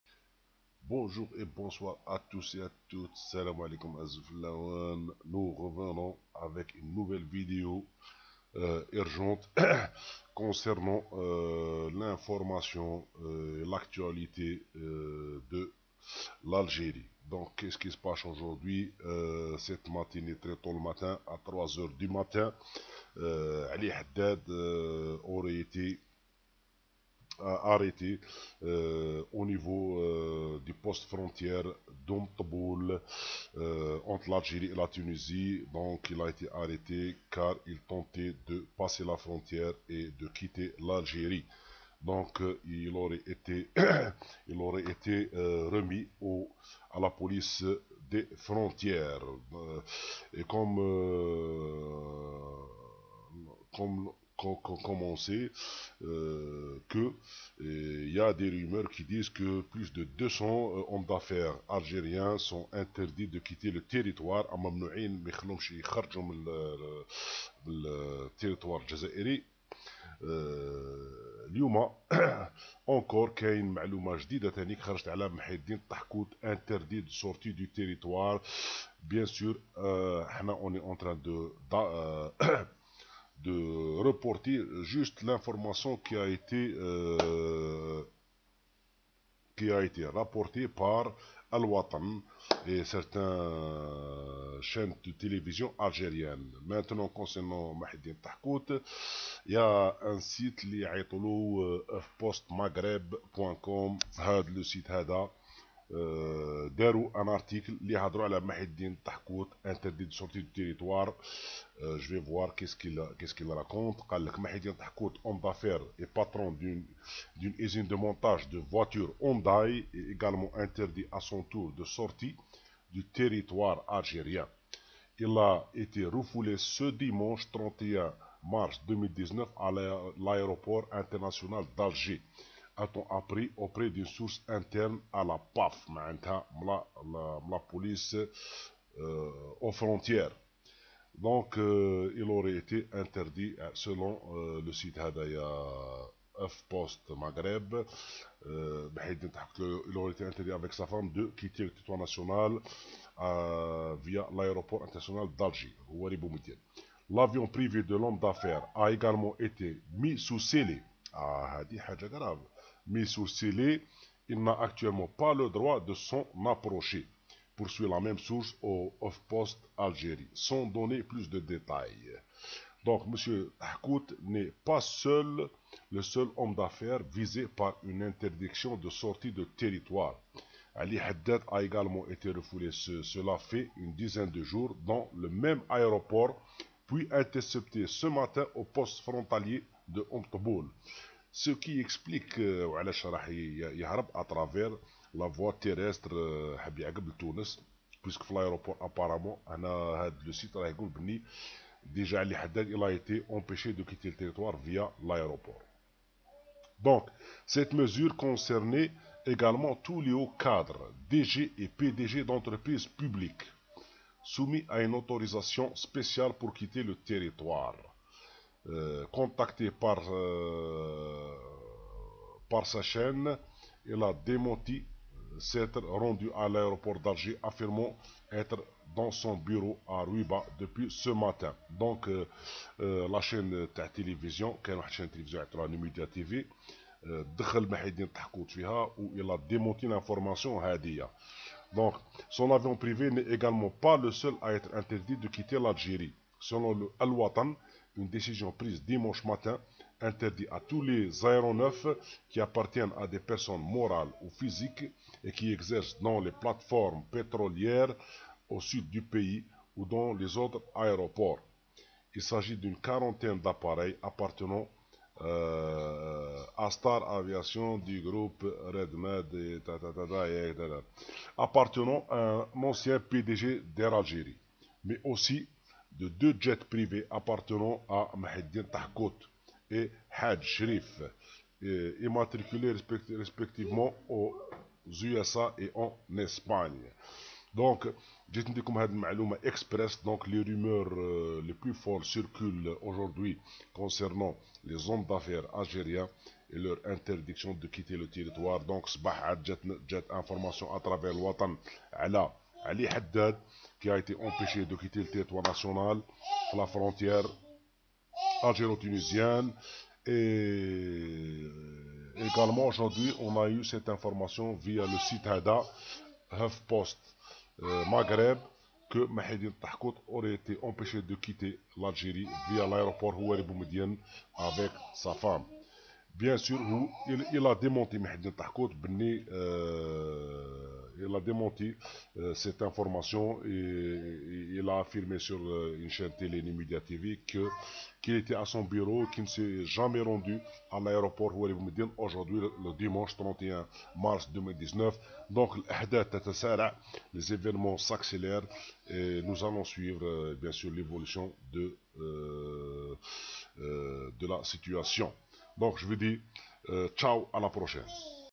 L'homme d'affaires Mahieddine Tahkout aurait été empêcher de quitter l'Algérie via l'aéroport
L'homme d'affaires Mahieddine Tahkout aurait été empêcher de quitter l'Algérie via l'aéroport selon un site internet d'informations